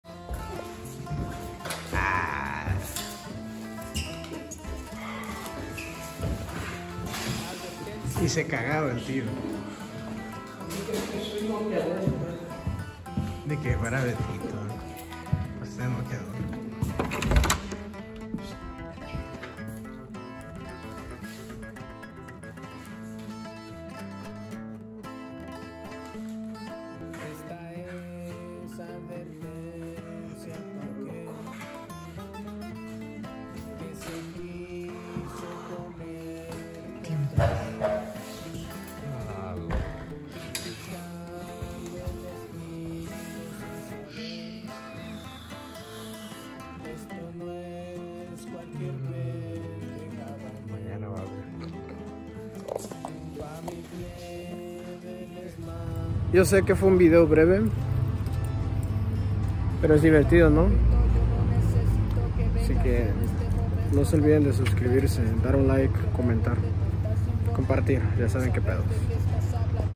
CARGANDO el PAJARITO con UNA MANO
Qué tal mi gente. En esta ocasión, estábamos caminando las escaleras en la casa, cuando mi tío levantó el scooter electrónico como si nada. Era un pajarito, ósea, un scooter electrónico. Es el de nosotros, es propio. Mi Tío Loko me pregunto, tu crees que no puedo levantar este pajarito? No supe si decir si o no. Y pues ya ven, es solo un video para que vean hasta donde llega mi tío y su fuerza, haha. Saben que esto es puro humor! Es del tipo de video que salen improvisados y de la nada. Nos vemos mañana mi familia linda ❤️❤️❤️.

⚡️Puedes Seguirme En Redes: 🌬💨🔥🌪